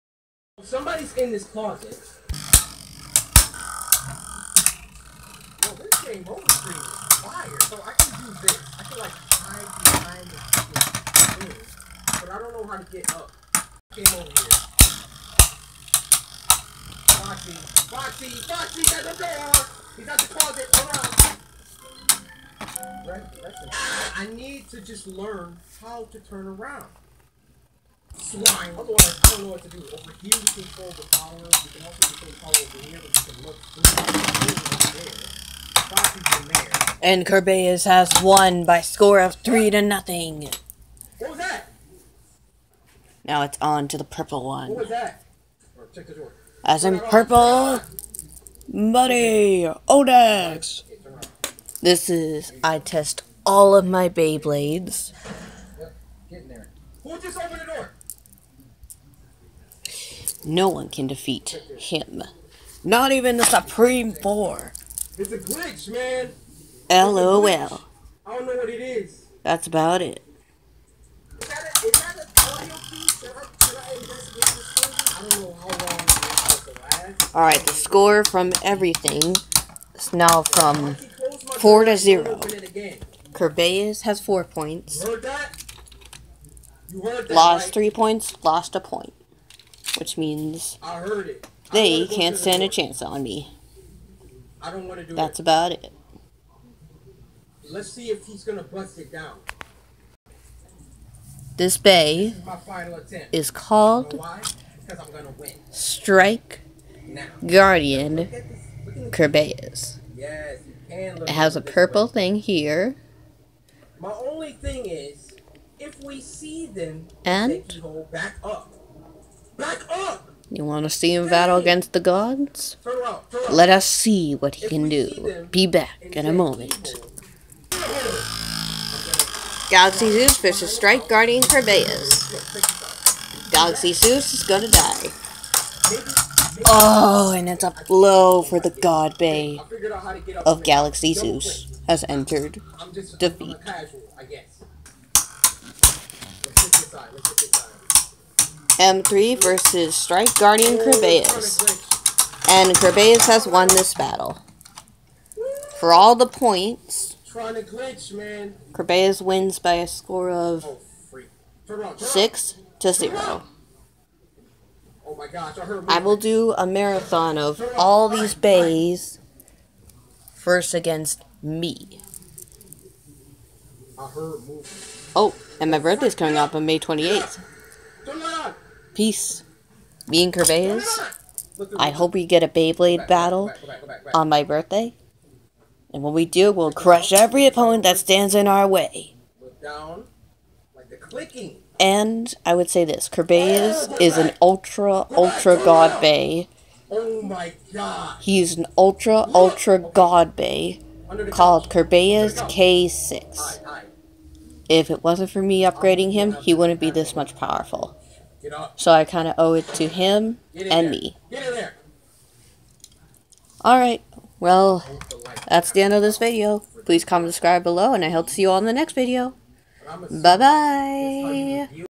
Ultra battle God kerbeus vs God's and Odax
Kerbeus is called strikes gaurdian kerbeus the ultra god bey  kerbeus K6 a balance type Beyblade.